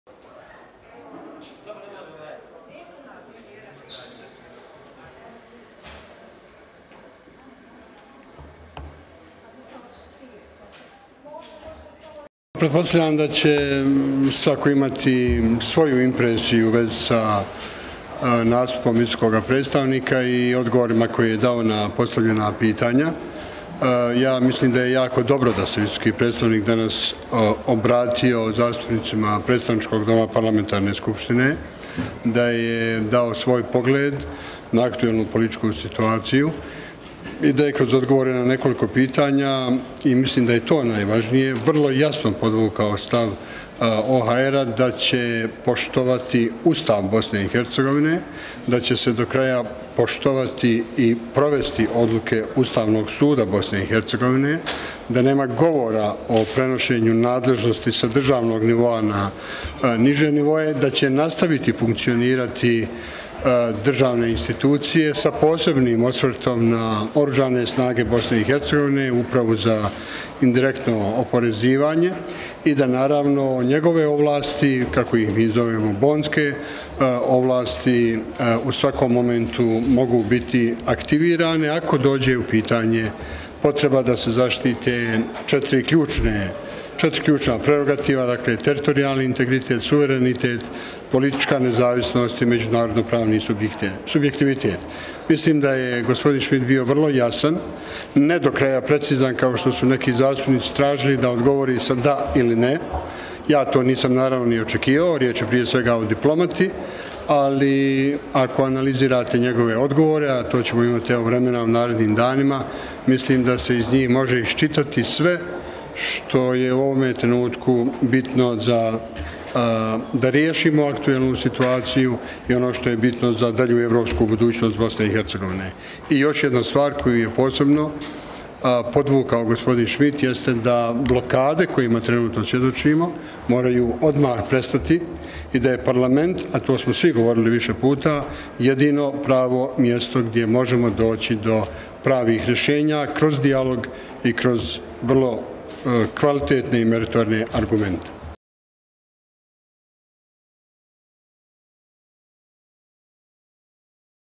Zvizdić: Schmidt je podvukao da blokade moraju odmah prestati
SARAJEVO, 28. oktobra (FENA) - Predsjedavajući Predstavničkog doma Parlamentarne skupštine BiH Denis Zvizdić izjavio je novinarima da je jako dobro da se visoki predstavnik međunarodne zajednice u BiH Christian Schmidt danas obratio poslanicima u Predstavničkom domu i da je dao svoj pogled na aktuelnu političku situaciju.